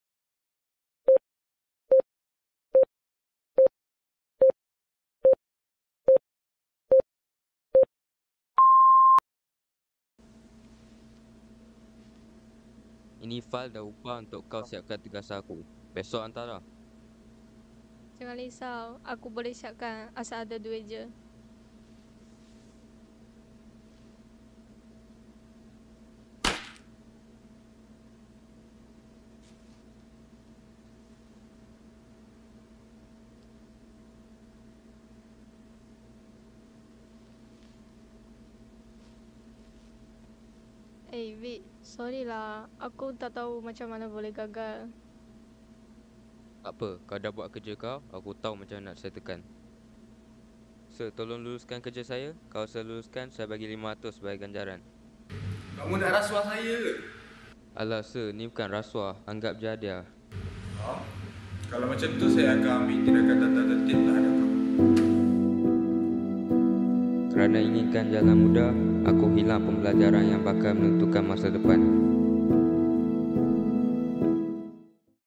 Public Service Annoucement_SUAPAN_DVV2023
Public Service Announcement akan memberikan impak yang besar kepada pelaku dan memberikan kesedaran kepada pelajar yang baru mula berjinak dalam aktiviti "suap-menyuap" ini bertujuan untuk berjaya tanpa usaha. Aktiviti rasuah ini dijalankan oleh pelajar bertujuan agar kerja yang diberikan oleh pensyarah dapat disiapkan secara mudah dan dapat melepasi semester tanpa gagal. Video ini juga memaparkan kesan atau tindakan yang akan diambil kepada individu yang melakukan aktiviti rasuah ini.

Kumpulan:
MUHAMMAD ISYRAFF HAKIEM BIN MOHAMAD SANI
KOVIT SRIPHAKDEE A/L CHAINARONG SUVANPRATUM
CHAN WAN LING
PUTRA MUHAMMAD FAREEZ
KUMARAN A\L SIVA SUBRAMANIAM